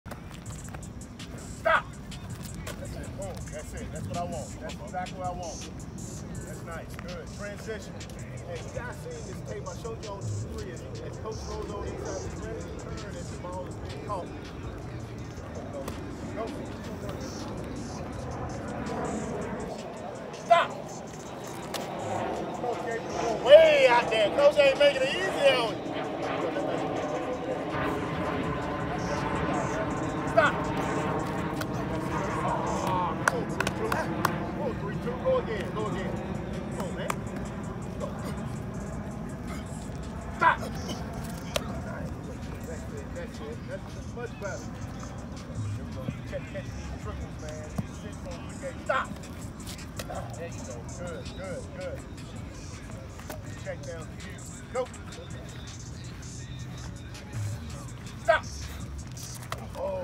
Running back drills at Commanders Minicamp John Keim Report
More highlights from the running backs group at Commanders minicamp.